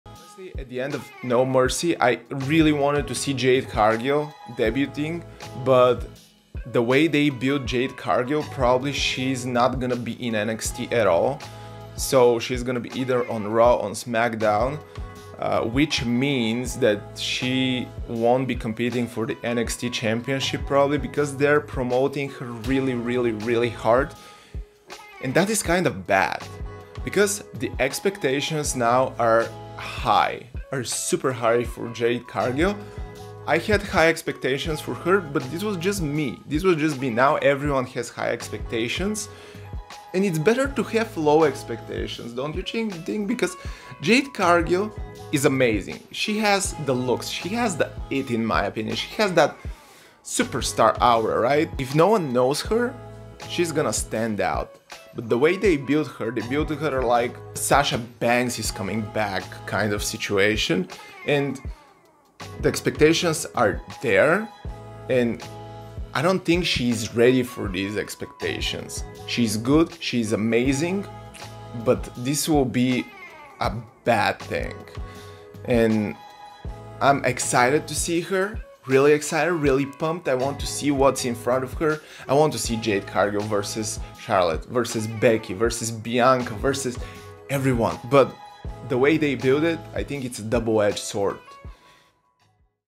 Jade Cargill's WWE Debut
This is what I think of Jade Cargill and the expectations attached to her.

☝️Vlogs: @Anzhelo
🤏Extra: @AnzheloExtra
💤Stream VODs: @AnzheloVods